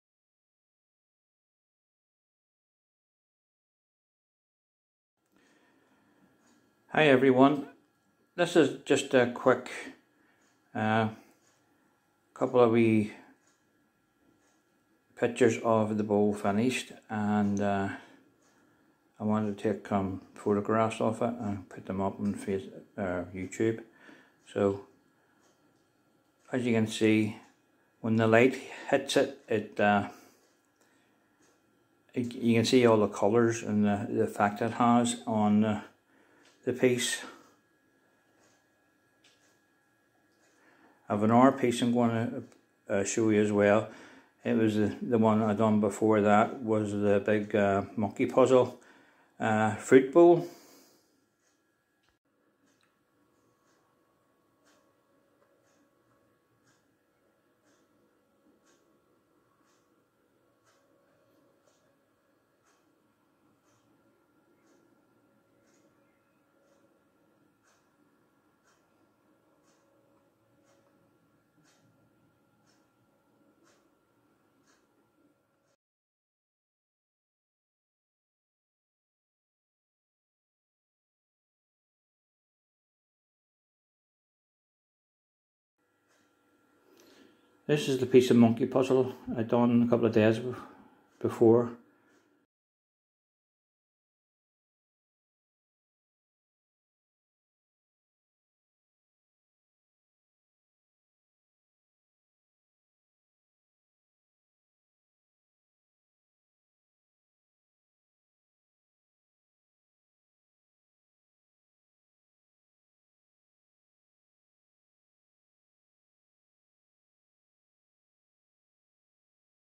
Maple Bowl with decorative rim.
This is a bowl made from a piece of Maple 9" wide by 1"3/4 deep and i am using jo sonja iridescent paints on the rim